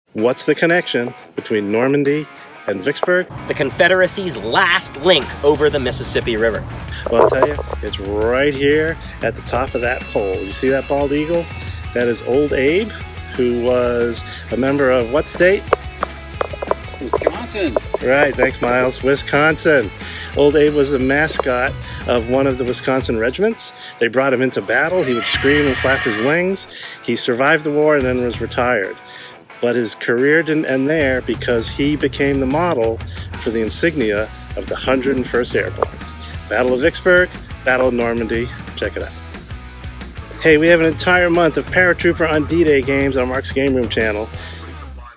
The Secret of the 150-year old Eagle!
Mark reveals the surprising connection that an eagle born over a century and half ago has to the modern U.S. military!  Even history buffs may be in for a surprise!